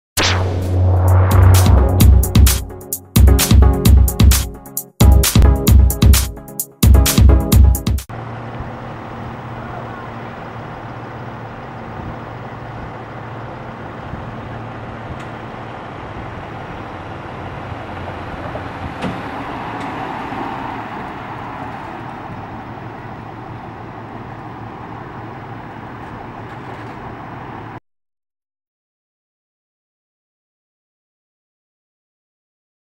C-K EMS - 1130 Arriving
Chatham-Kent EMS, 1130 responding and arriving on scene of a two vehicle MVC on McNaughton Ave. E. in Chatham.